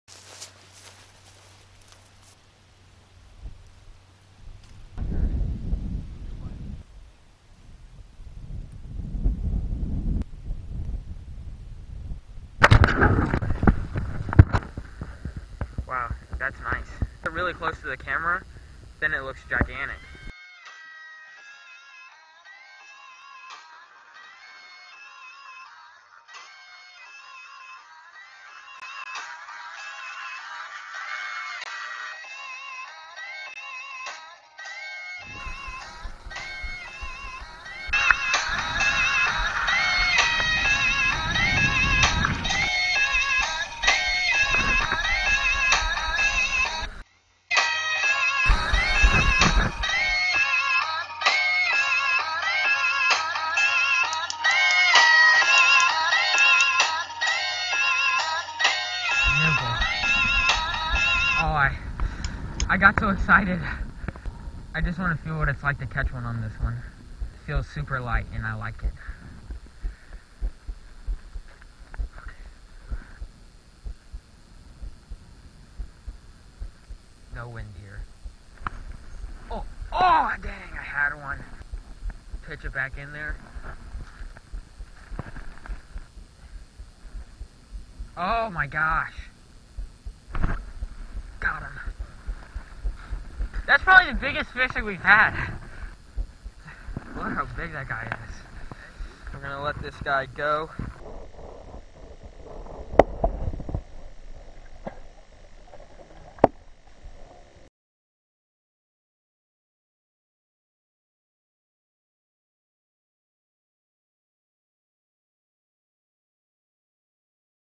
Only FLAIR fish
sorry for the audio problems. I will fix them and re upload soon.
all fish were lmb or largemouth bass.